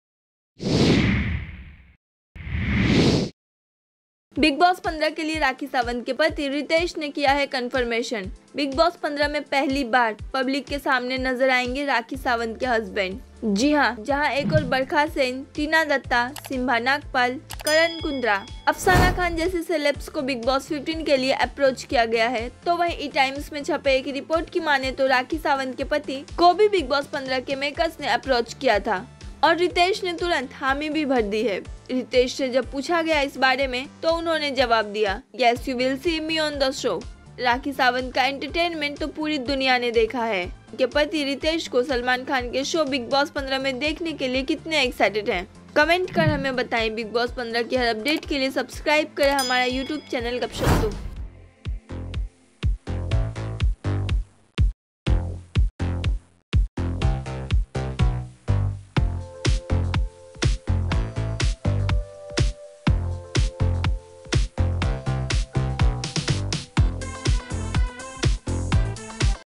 Rakhi Sawant Husband Ritesh CONFIRMED on Bigg Boss 15 | Salman Khan ke Bigg Boss15 me aayenge Ritesh
Bigg Boss 15 me aayega Rakhi Sawant ka Husband Ritesh.